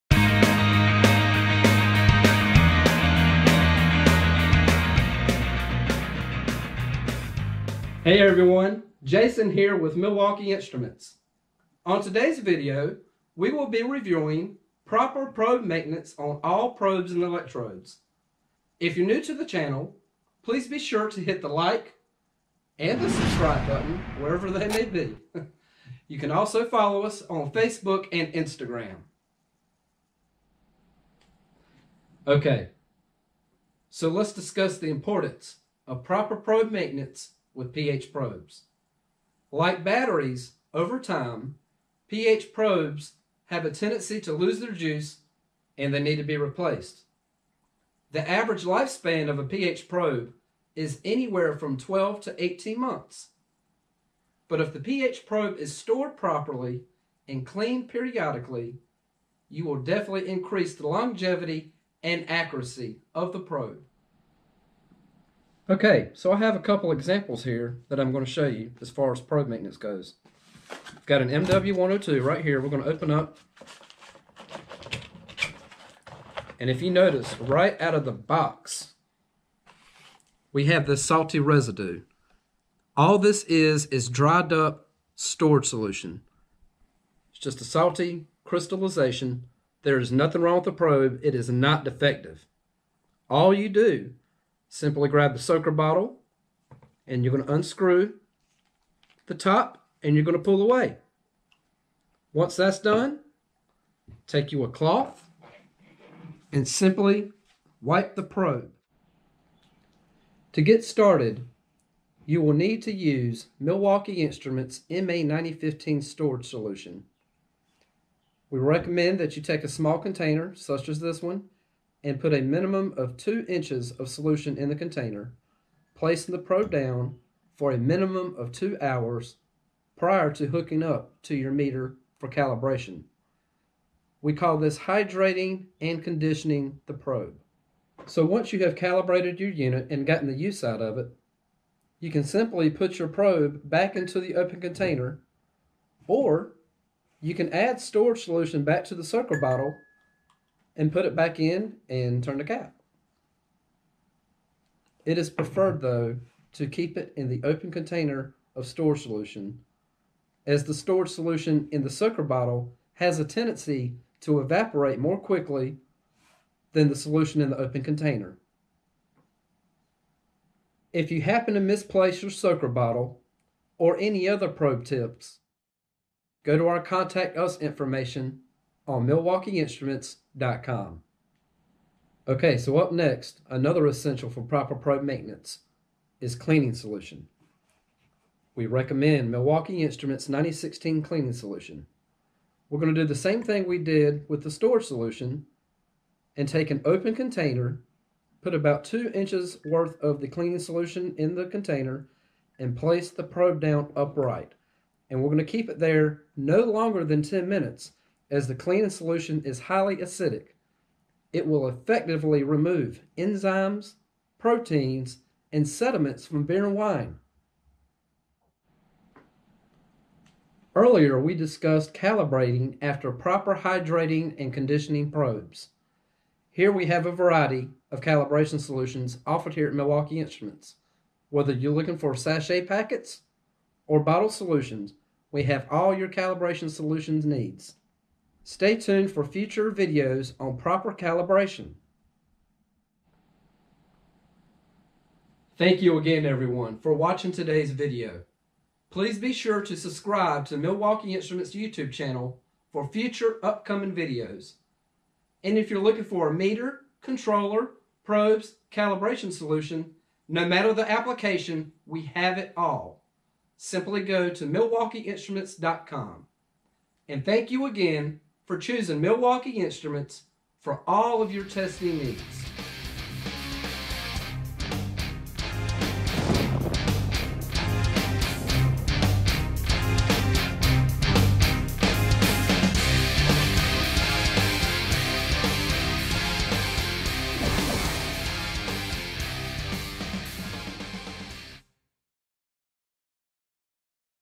Milwaukee Instruments Probe Maintenance
This video explains the proper way to maintain your Milwaukee Instruments probes for long life and accuracy.